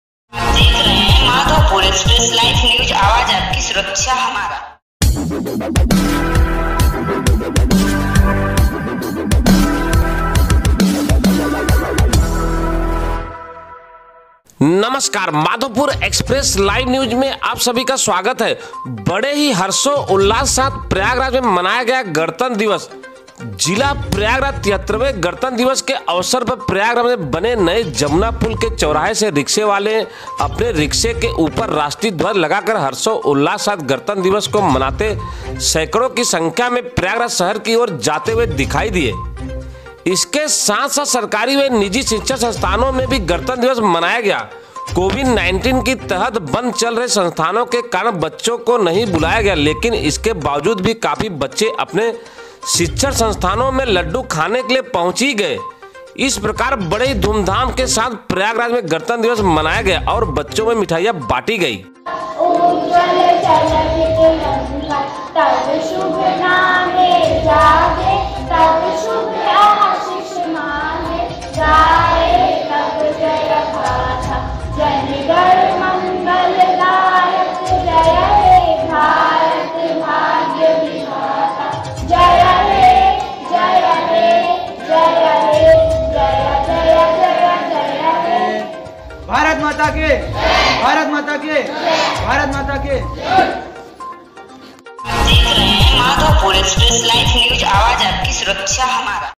प्रयागराज गणतंत्र दिवस के अवसर शिक्षण संस्थान और रिक्शा वालों ने मनाया हर्षोल्लास के साथ
माधोपुर एक्सप्रेस लाइव न्यूज़ राजेंद्र पांडे